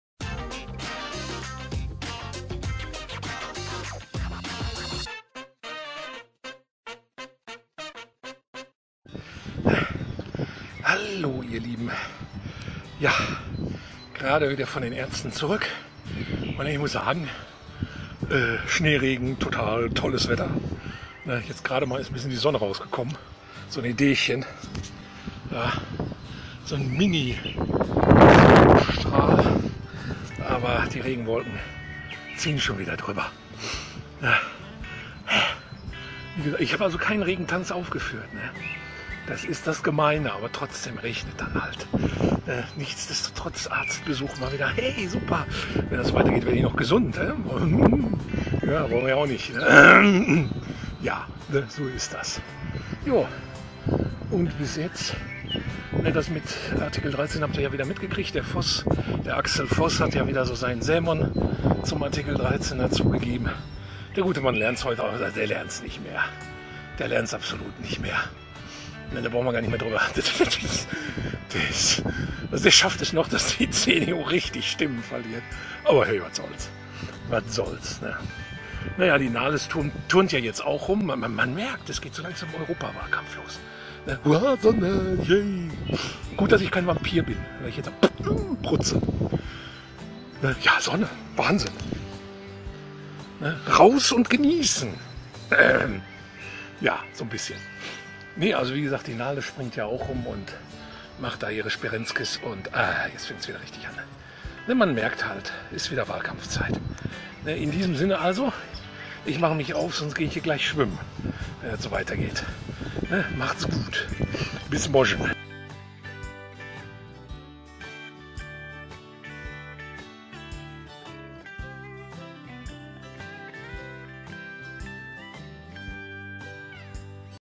RollatorMän unterwegs 14 03
Hallo,

und heute mal unterwegs, obwohl man bei dem Wetter keinen Hund vor die Tür jagt, ist RollatorMän Wetterbeständig und Feuerfest naja wenigstens Wetterbeständig.

Viel Spaß
THSAR aka RollatorMän